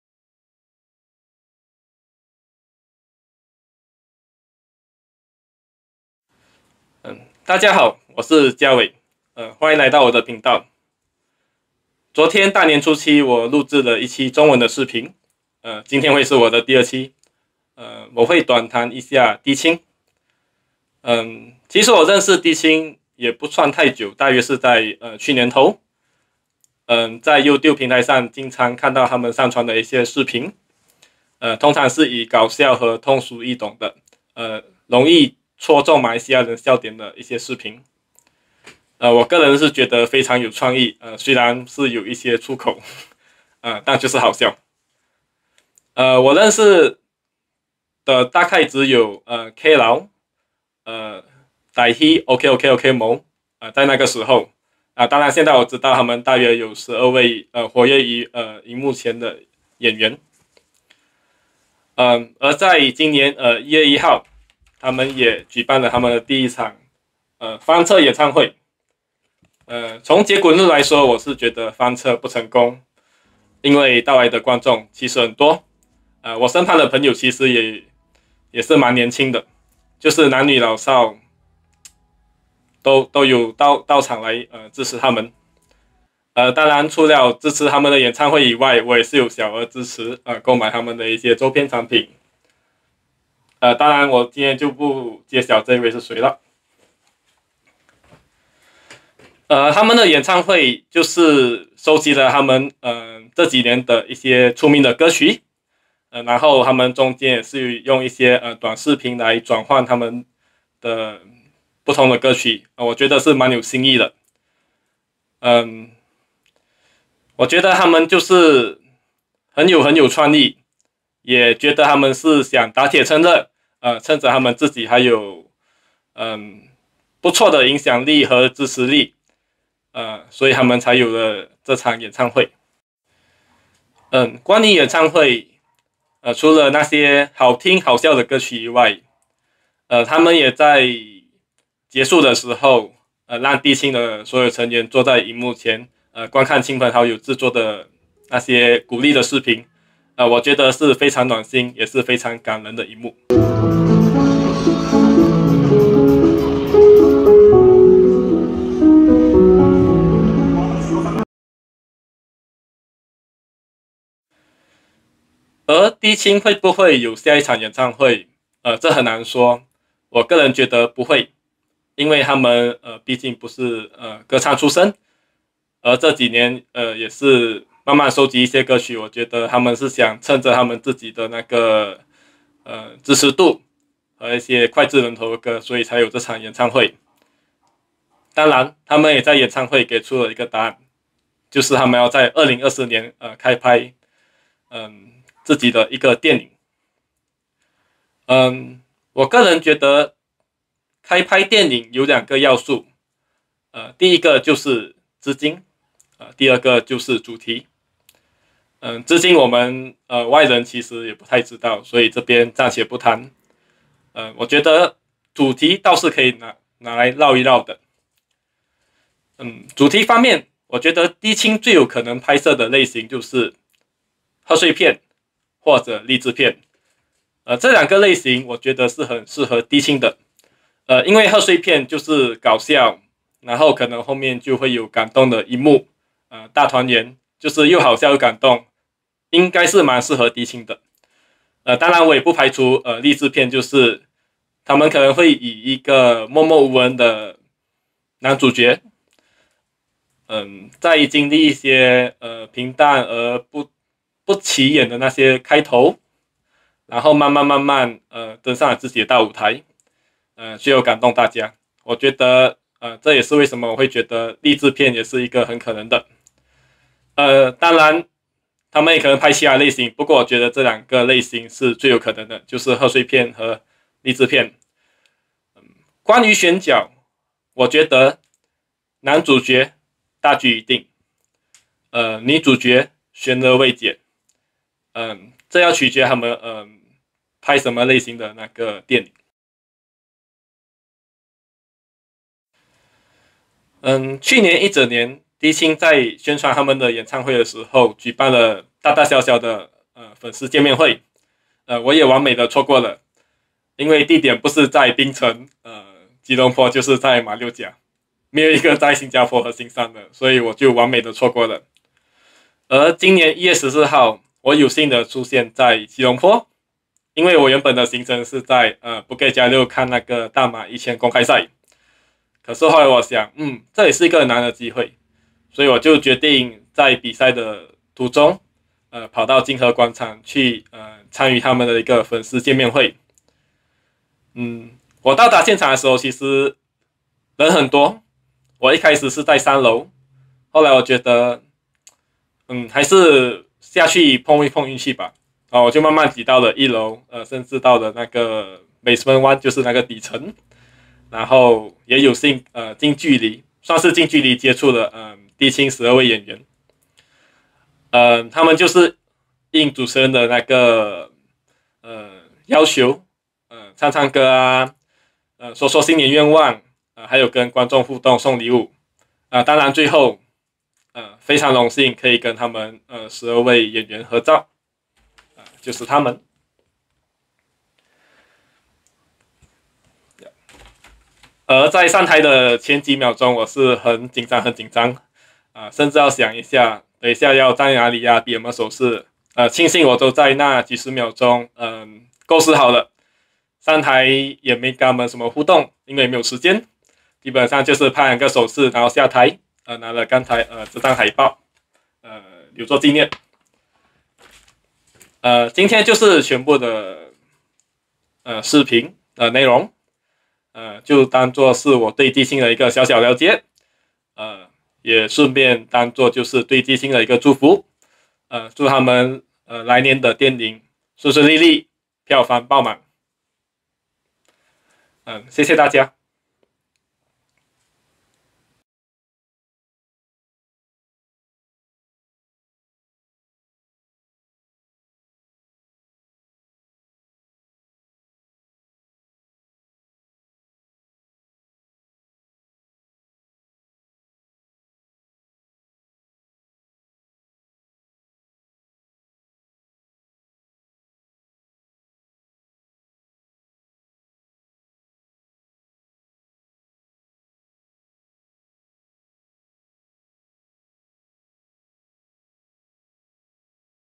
听听就好2—短谈低清—29012023
这是我以中文讲解的第二期视频。去年2022年，我在每一个月，用英文讲解一个课题，就是Inspire You and Me，这算是去年的延续，只是换了一个语言。我取名为“听听就好”。
This is my second video spoken in Mandarin. Last year, I recorded a video in English every month, the series is Inspire You and Me. So, this is the continuation of this series, it is just another language. This series is "Ting Ting Jiu Hao", (meaning just listen only, and don't take it too seriously)

这一期我会短谈低清，他们在Youtube平台上有很多好笑的视频。我也会短谈他们在一月的演唱会和粉丝见面会。
In this video, I will talk about Dissy who is producing fun videos on Youtube. I will also talk about their concert and meet up session with fans in January.